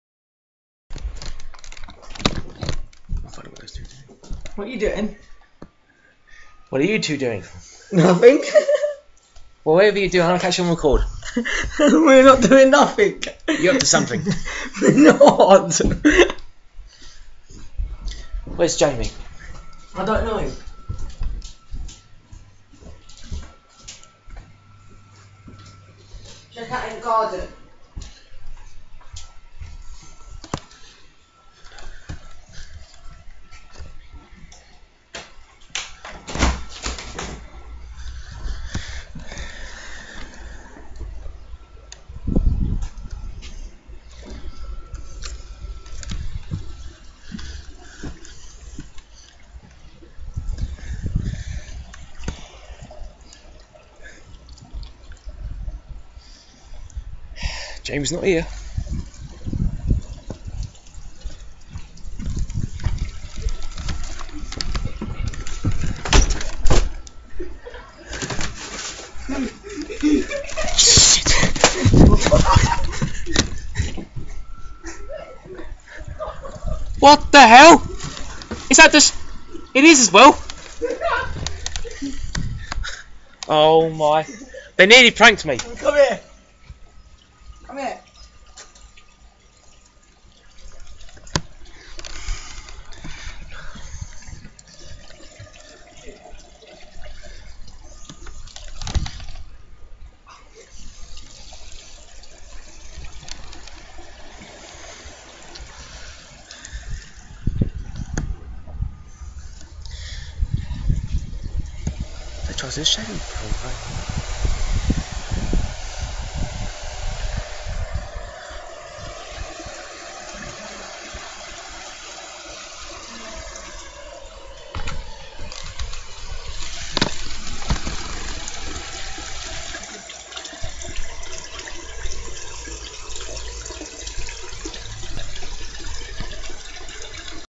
My 15th Prank- Louis used SHAVING FOAM IN CRISP PACKET PRANK(i nearly fell for it folks)Sorry failed
i nearly fell for the prank,but this was a fell prank attempt by Louis
SORRY BRO
MAYBE NEXT TIME,U WILL GET ME,BUT NOT THIS TIME HAHAHA